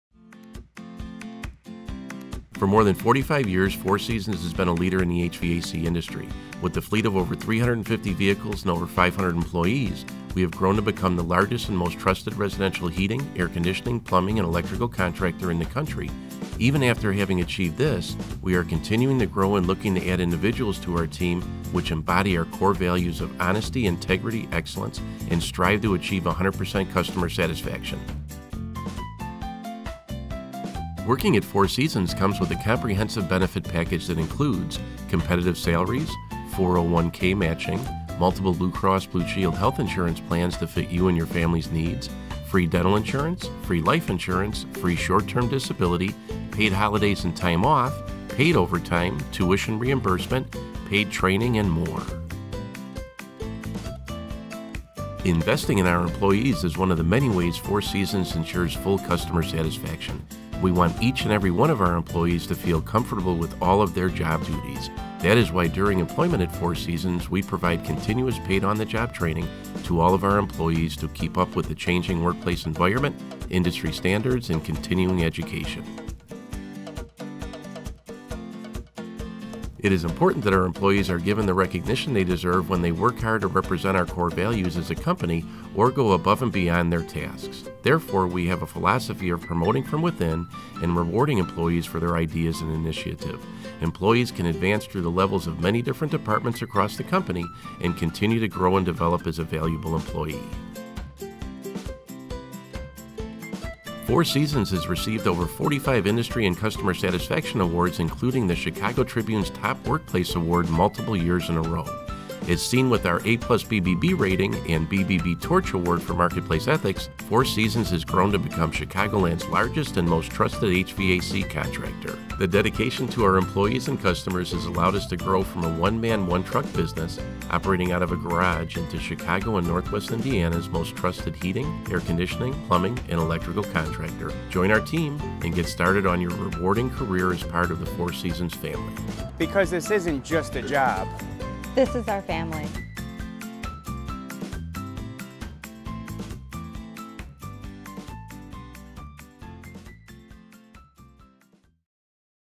Why Work at Four Seasons
Four Seasons Heating, Air Conditioning, and Plumbing is the leading HVAC company in Chicagoland with over 500 employees. We are always looking for qualified professionals for our office, sales, and HVAC technician, installation, and repair positions. With excellent employee morale, Four Seasons seeks to create a positive work environment and always strives to set each worker up for success in their chosen career.